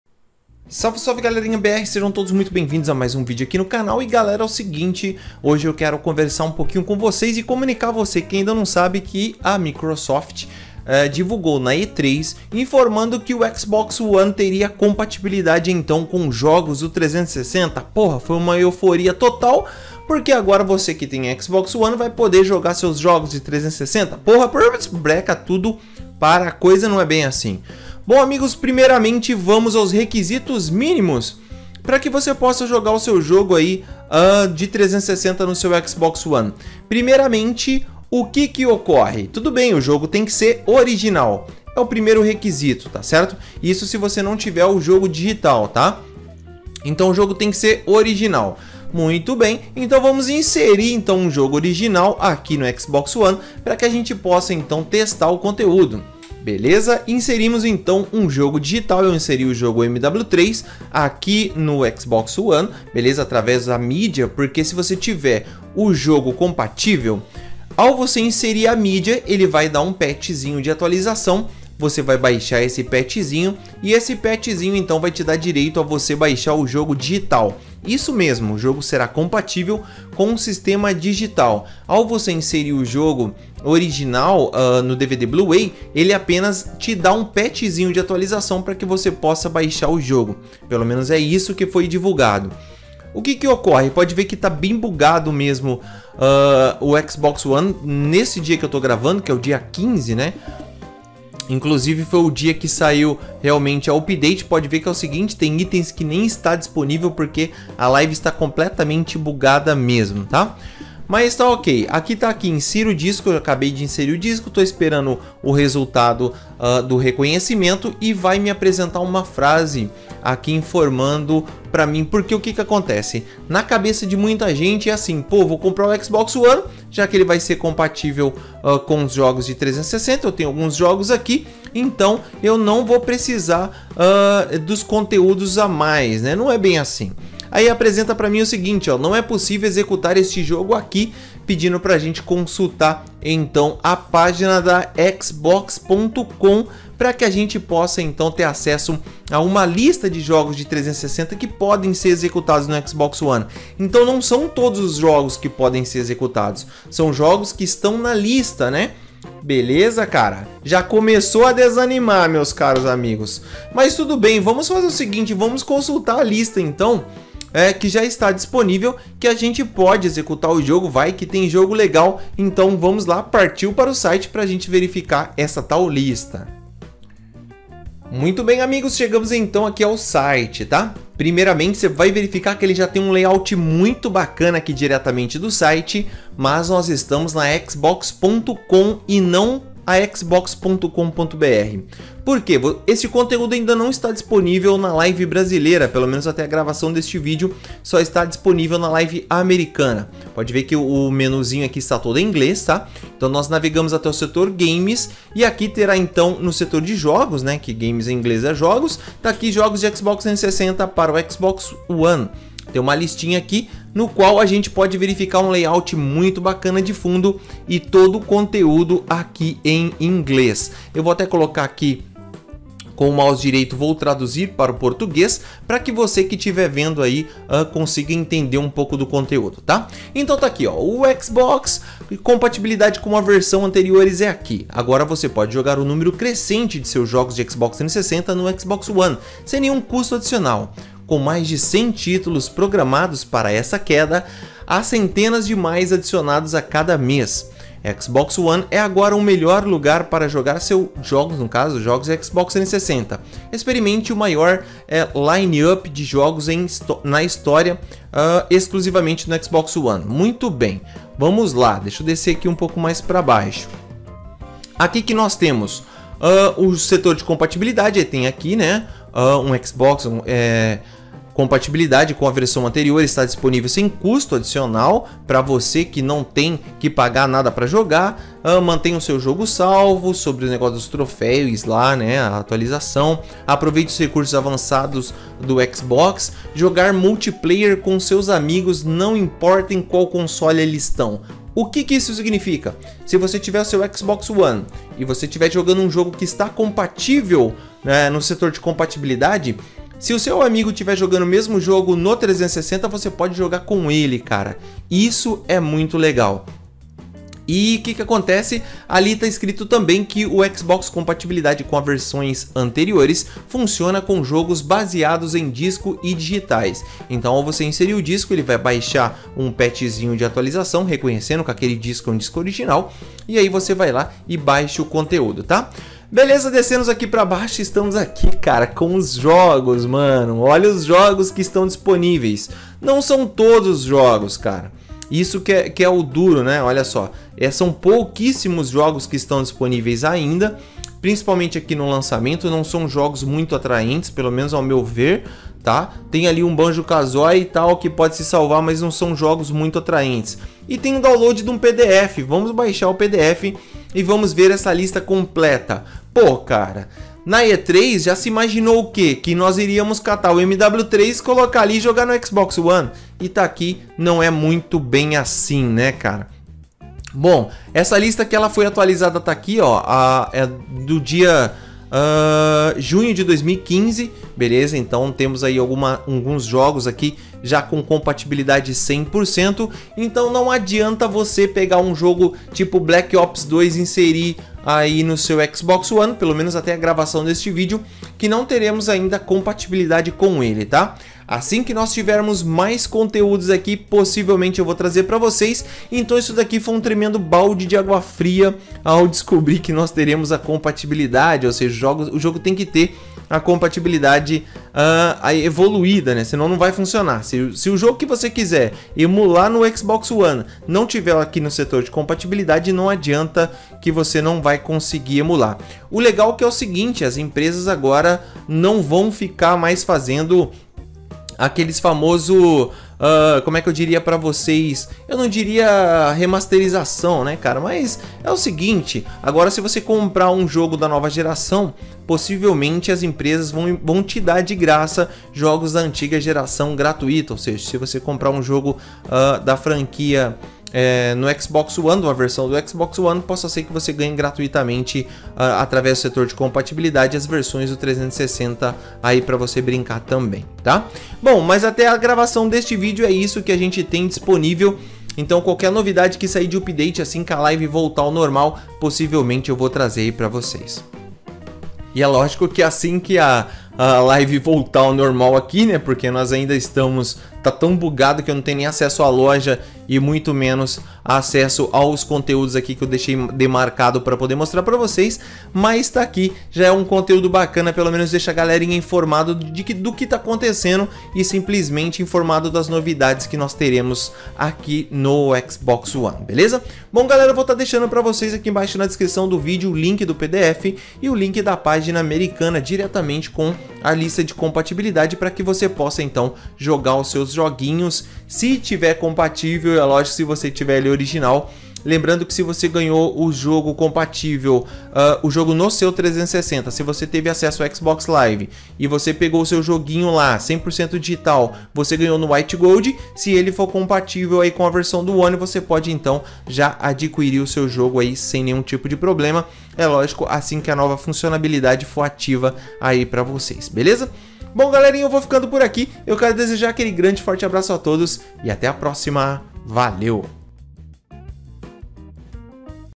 [ ONE ] • [PREVIEW] - Jogue jogos de Xbox360 no Xbox One
.Veja abaixo as Playlists do canal : Rubens Yama
Conheça nosso trabalho um pouco mais
Os jogos serão acrescentados conforme a
liberação de Update para os mesmo, portanto
esta lista abaixo tende a crescer a cada dia.
Paginas com a lista de jogos
do xbox 360 compatíveis com Xbox One abaixo :

Boa sorte a todos e ate a próxima valew